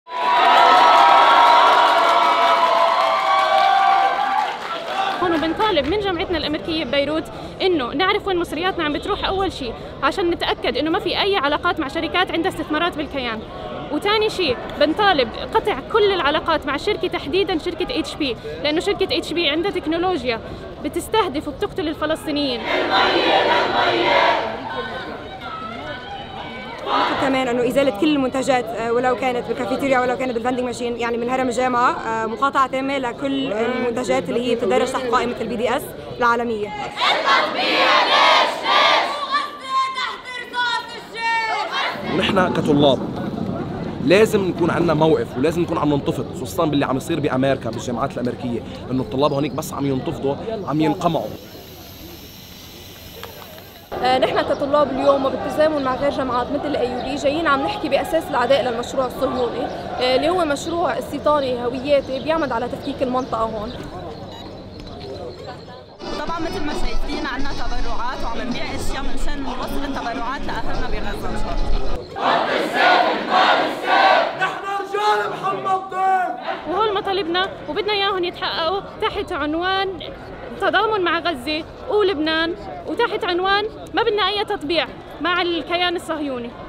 جامعات لبنان تتحرّك
هكذا كان تحرّك طلّاب الجامعات في لبنان اليوم، تضامناً مع غزّة، وضغطاً على الإدارات للتقيّّد بالشفافية والمقاطعة في استثماراتها.